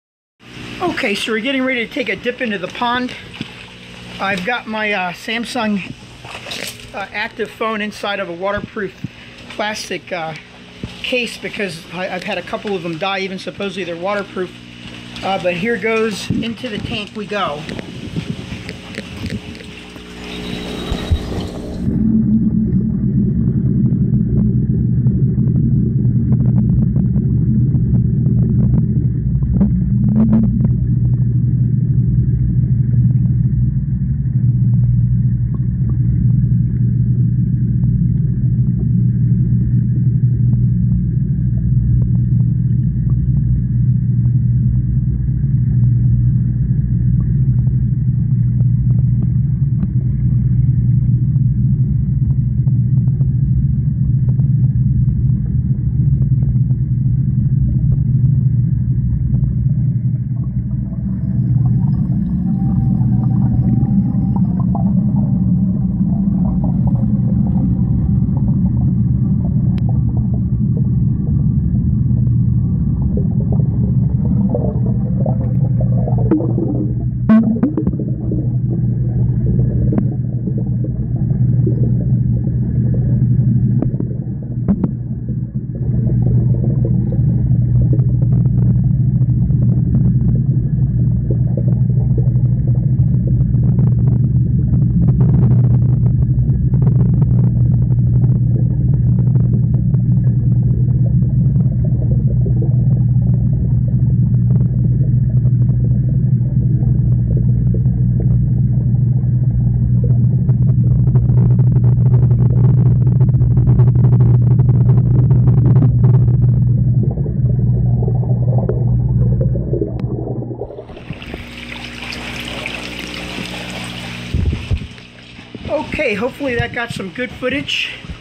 Dip in the pond and underwater catfish motel 21 APR 15
Aquaponics update 21 APR 15